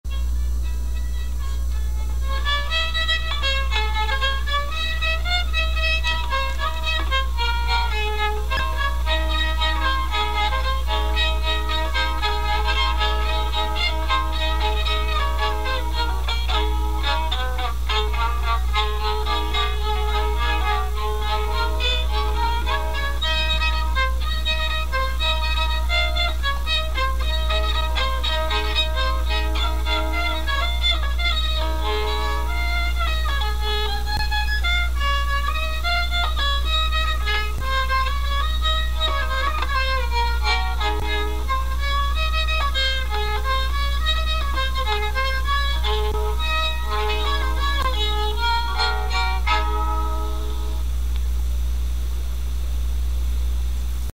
Winnie + Dulcimer Fiddle
My instrument of choice is the mountain dulcimer - on my lap, at right.
But - with a bow - I can also make it sound like an olde-tyme fiddle - and I did this after just a few hours (years ago).

Takes a lot of work and resin on that treble string nearest me me (and the whole thing is not 'curved' like a violin) - it's not something I'd do a lot of at event gigs...but it's an offering!